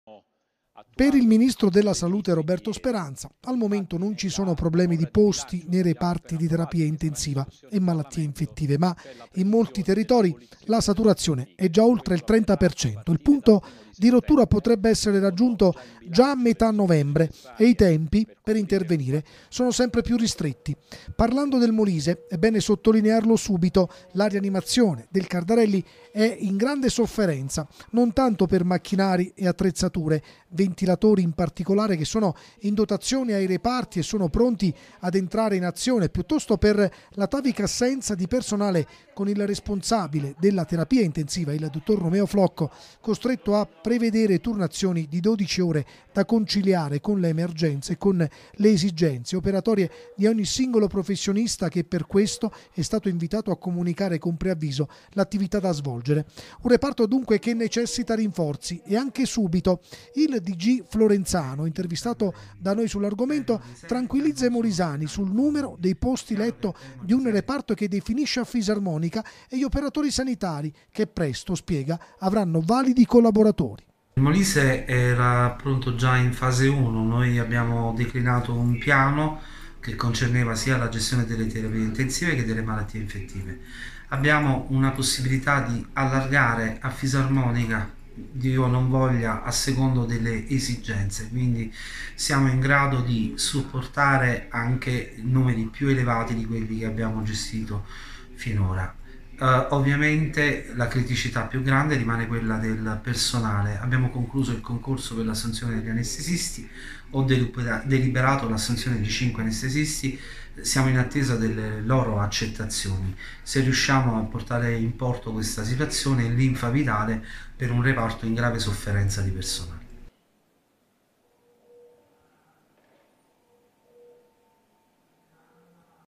Rianimazione del Cardarelli in grande sofferenza. Florenzano (dg Asrem): in arri...
Servizi TG | Rianimazione del Cardarelli in grande sofferenza. Florenzano (dg Asrem): in arrivo cinque anestesisti | 2/11/2020